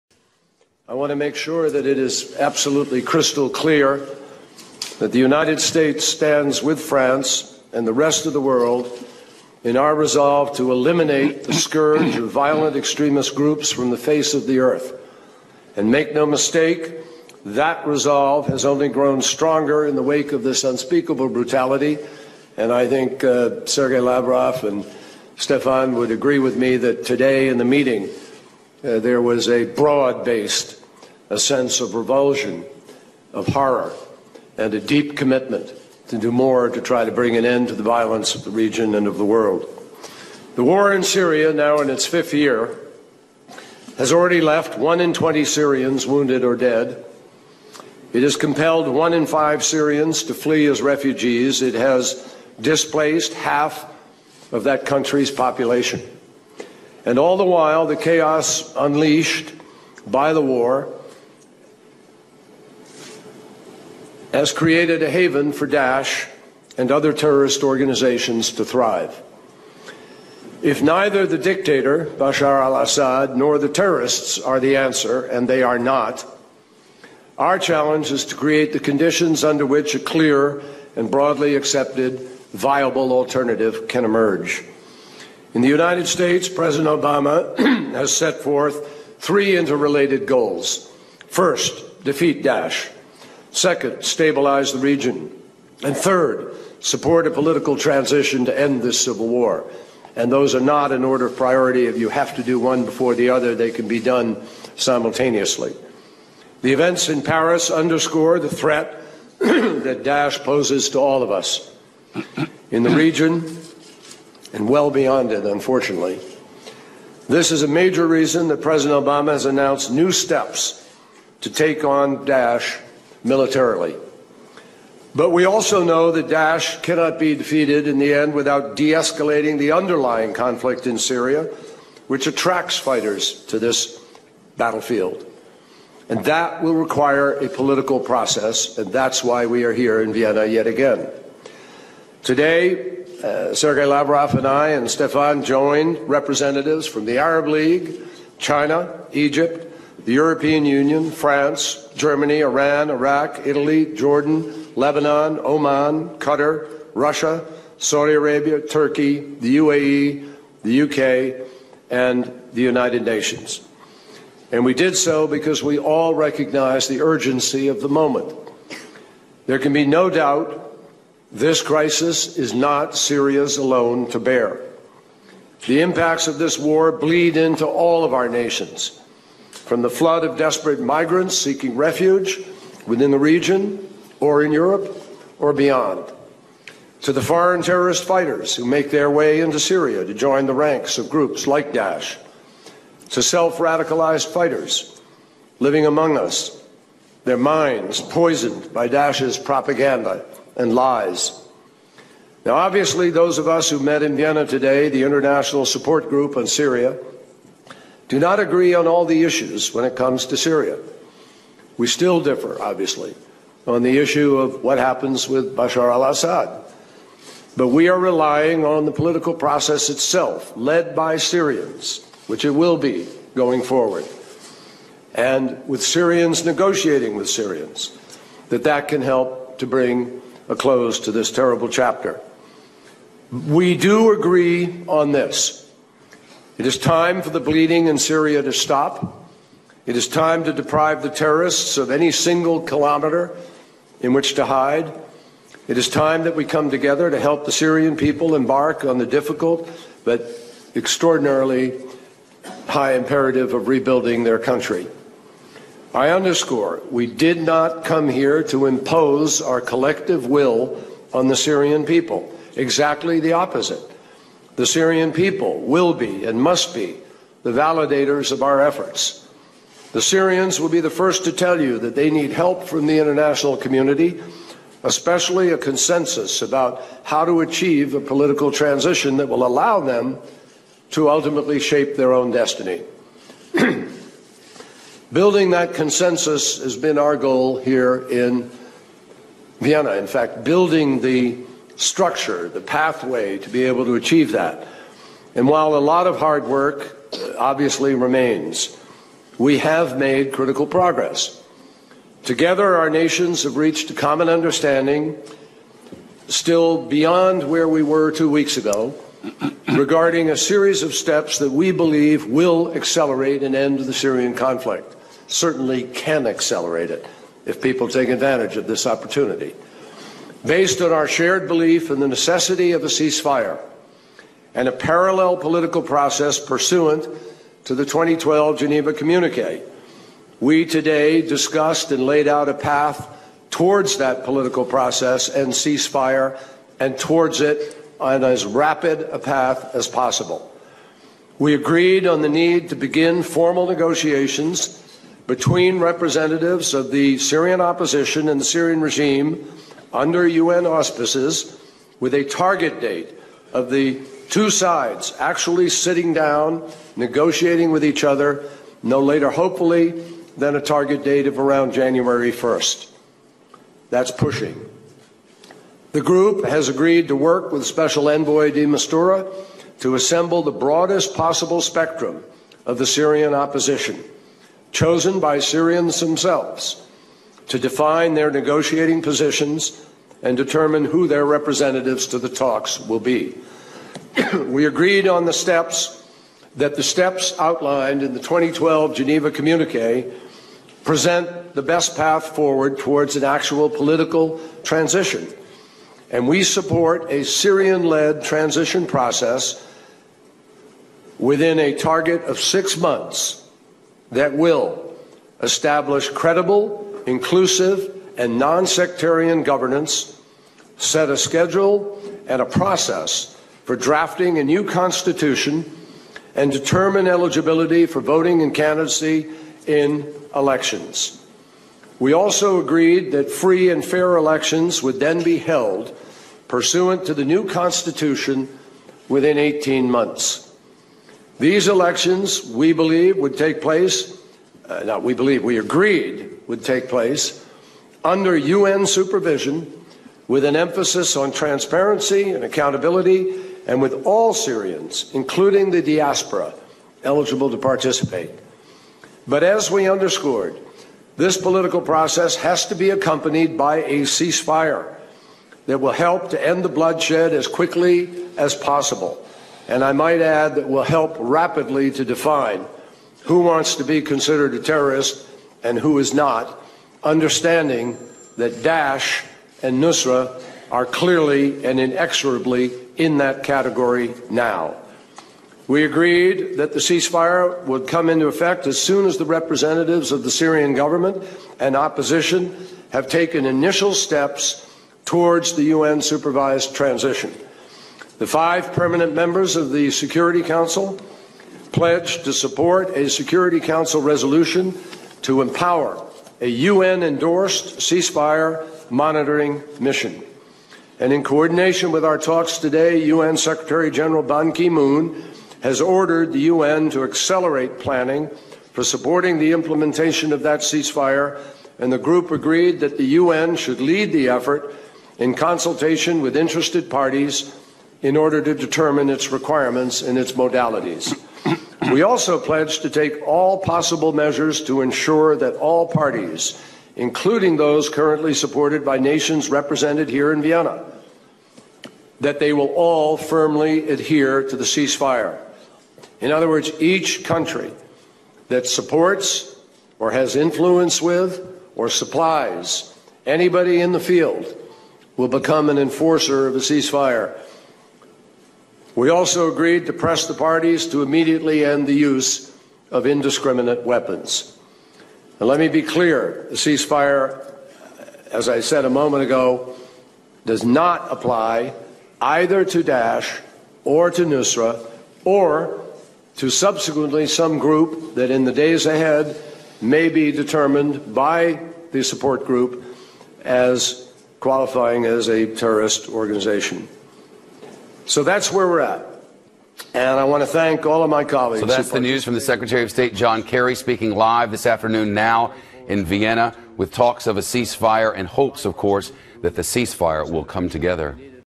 RickWells.US - John Kerry Announes Plan To Turn Syria Into Iraqi or Libyan Success Story
John Kerry begins his announcement with a statement of a new, more serious approach to fighting ISIS, a supposed result of the brutal attack they launched in Paris days earlier. The US is now committed, he says, to eliminating all “violent extremists groups.” The use of code words for Islamic terrorists is required in a regime that has handcuffed itself by political correctness. It’s a perverse one in which the prolific butchers of Islamic State are granted status equal to that of a lone gunman who murdered nine innocent people in a Charleston church. They are also granted protection from being identified as who they are.

After listing some statistics of the negative impact the civil war in Syria has had on its population, including the creation of a safe haven for ISIS and other terrorist organizations, Kerry begins to detail the outline for solutions the group of nations has agreed to implement. He first asserts that the long held contention that Assad must go remains, depicting his regime as being on one end of the spectrum and the equally unacceptable terrorists on the other. Kerry states the next step for the new nation building cabal, “Our challenge is to create the conditions under which a clear, broadly accepted and viable alternative can emerge.

Having just unraveled the success of their predecessors in Iraq and destroyed the stable Libya which existed under Gaddafi, it’s amazing that their heavily-veiled true motivations for demanding the exit of Assad are being advanced. Perhaps the chaos of Libya and Iraq are exactly the “solution” they are seeking. Maybe they’re still backing their alternative terrorist-lite group to replace Assad, such as the unknown “Rebels” that have received so much money and produced so few results. Maybe they’re attempting to wedge the Muslim Brotherhood somehow into the picture, having lost ground in Egypt when Obama and Kerry couldn’t overcome the will of the people. Whatever their secret motivations, we’ll unlikely ever be told the truth by these people. They’ve only got a little over a year left in power to destabilize as much of the region as possible.

Please visit RickWells.US